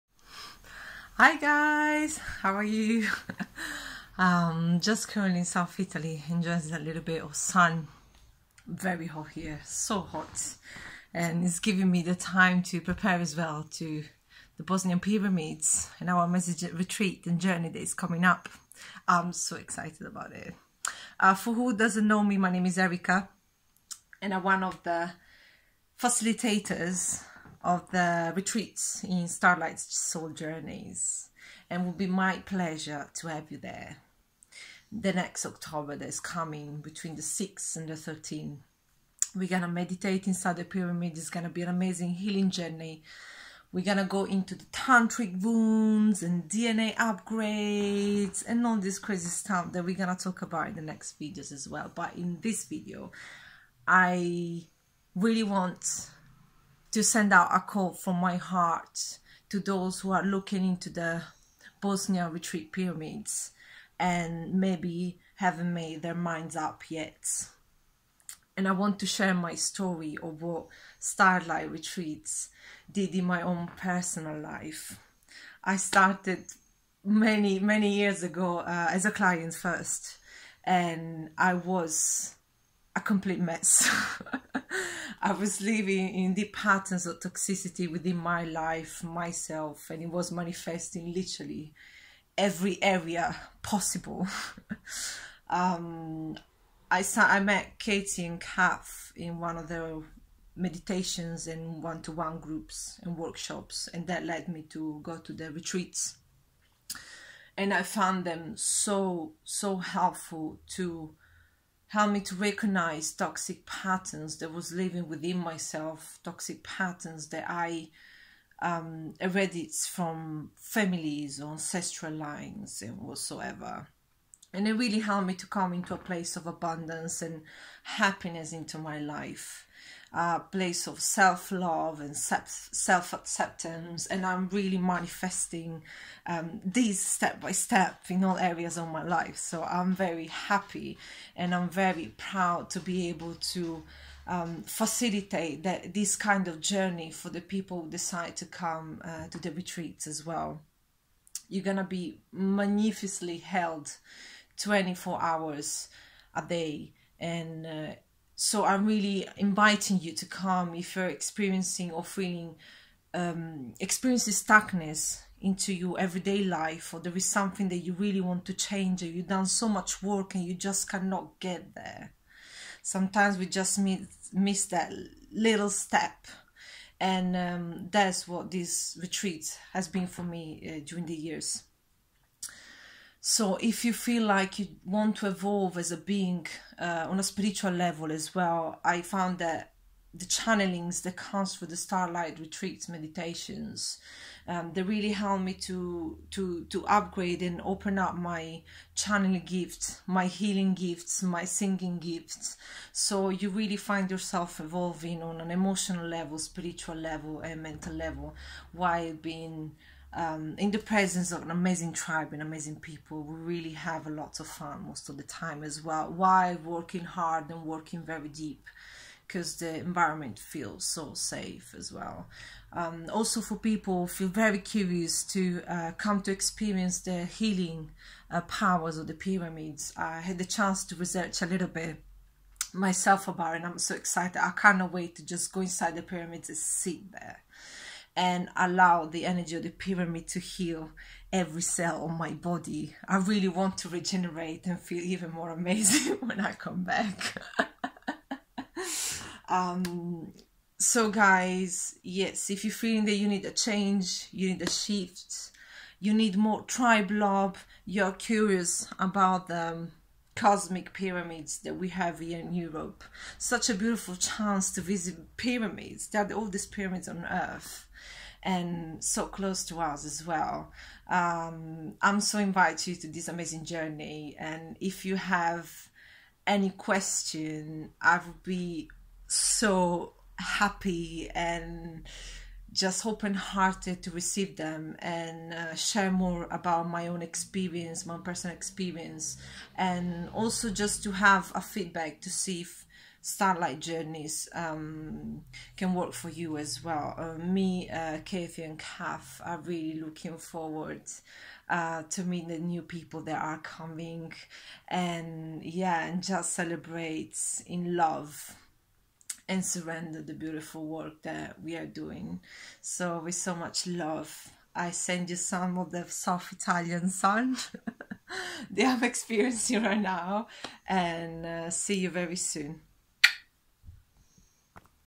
Erica's Starlight Story....
Erica now a starlight space holder on all of our soul journeys overseas started as a client with us- here she shares the story of her journey with us and talks a bit about starlights upcoming journey to Bosnia